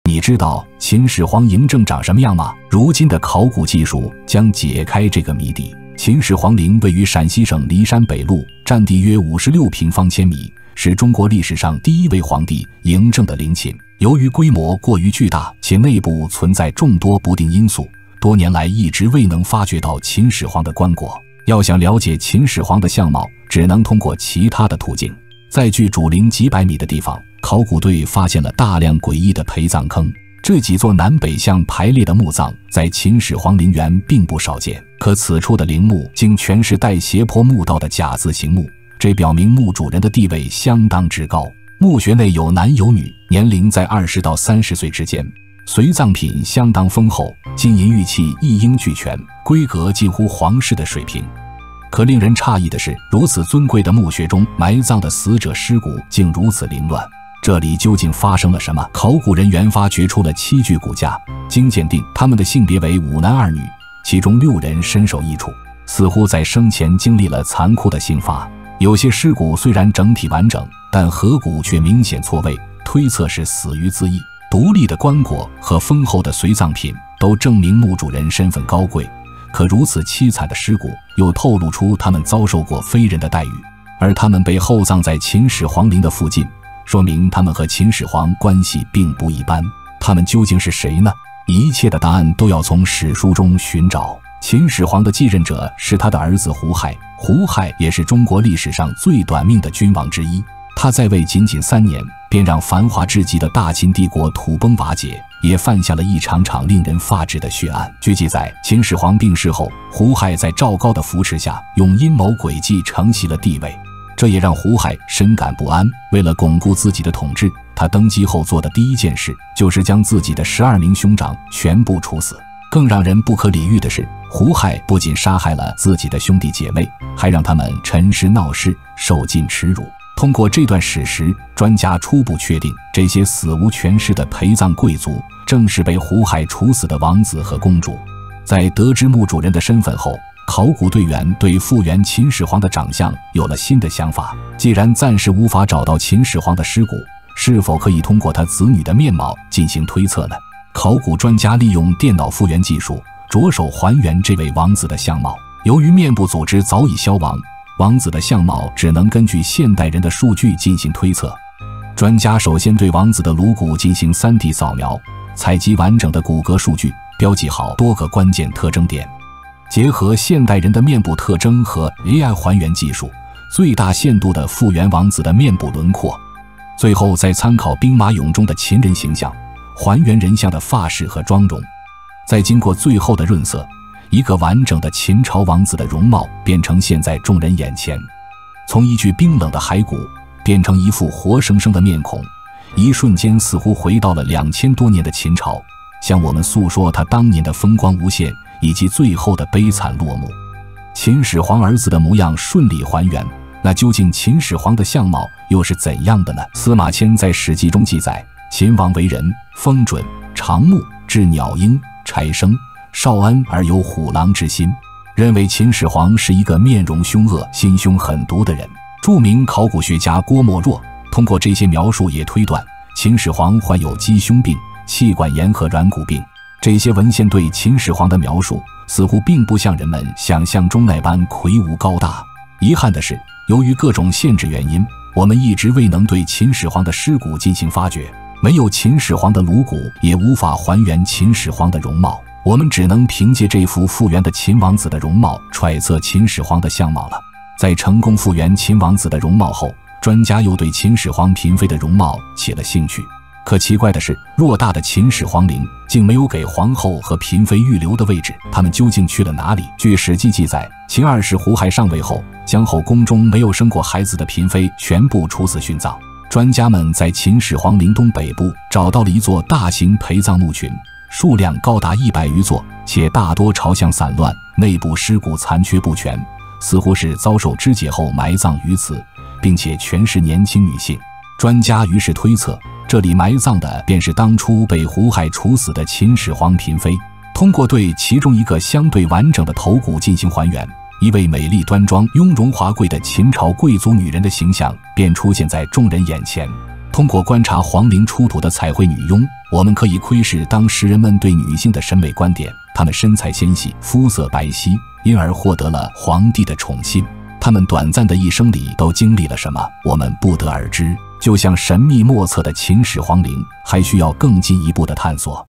【完整版解說】99%的人都不知道！秦始皇竟然長這樣？考古
你知道秦始皇嬴政長什麼樣嗎？為什麼秦始皇的兒女全部死無全尸？後宮妃子也被肢解亂葬？如今的考古技術將解開這個謎底……